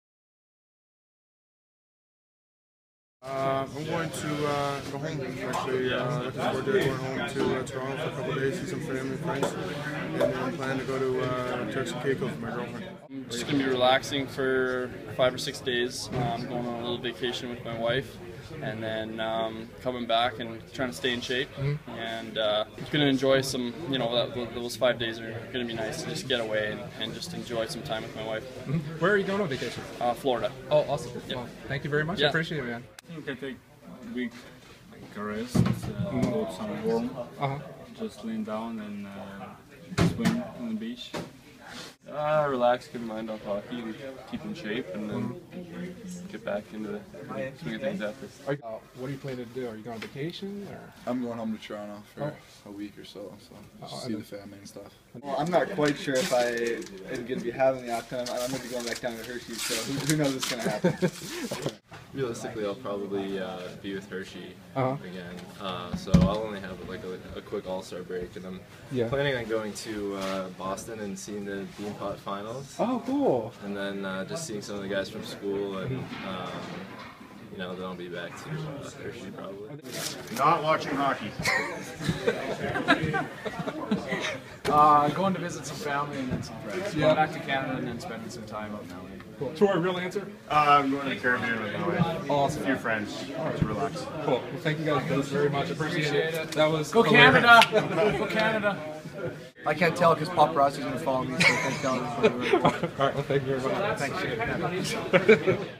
Caps Players Share Their Olympic Vacations
We speak with Jay Beagle, Joel Ward, Brooks Laich, Jason Chimera, Troy Brouwer, Braden Holtby and more about how they're spending their Olympic breaks.